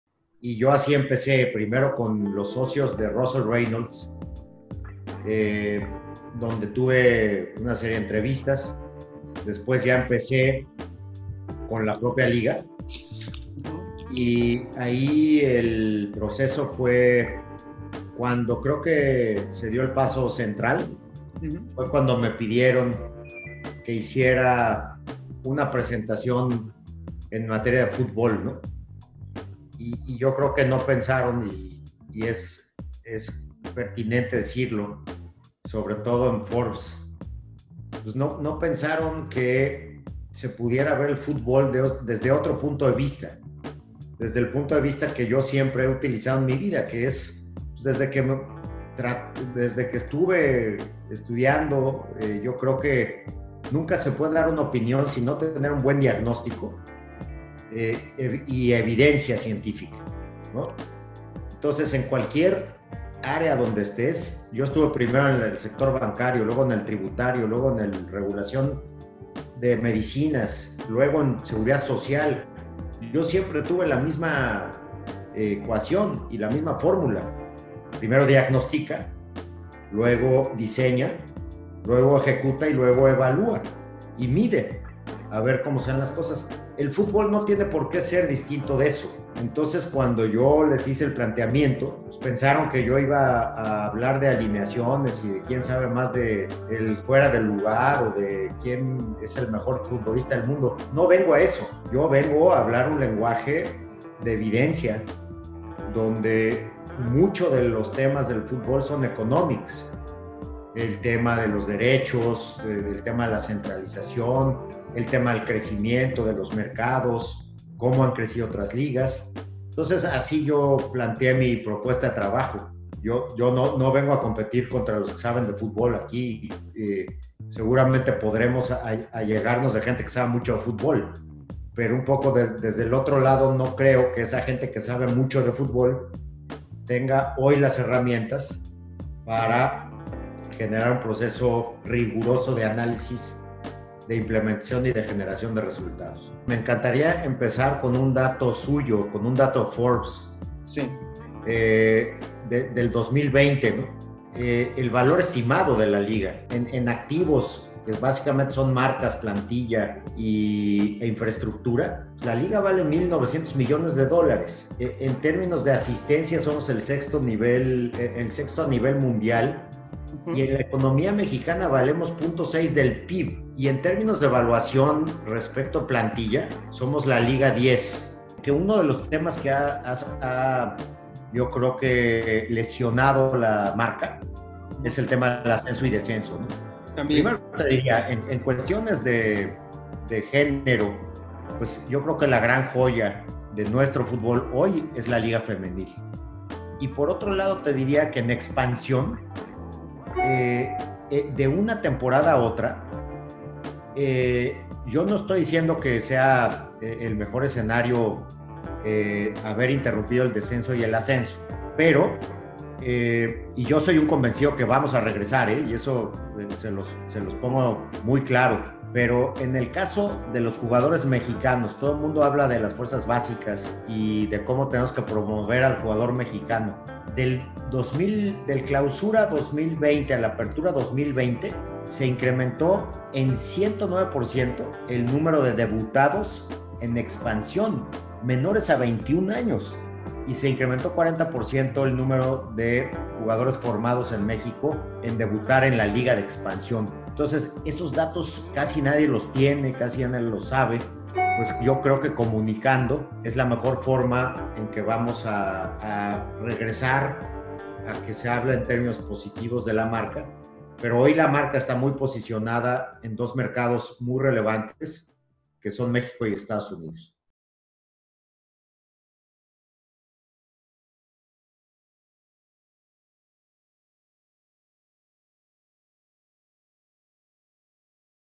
Esta es la ruta de Mikel Arriola para generar ingresos en la Liga MX
Mikel Arriola afirma que la marca Liga MX “es el activo más valioso que tenemos y hay que cuidarlo”.